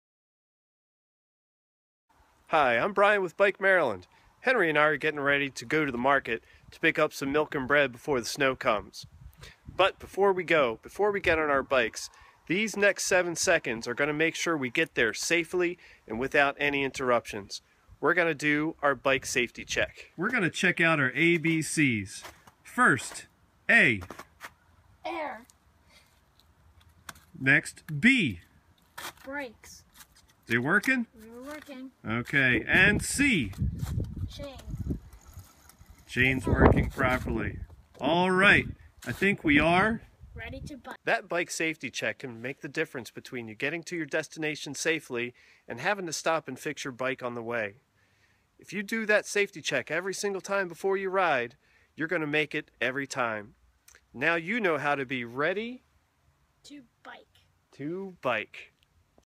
ABCs - Air, Brakes, and Chain
This short video describes how to perform a bike safety check before a ride.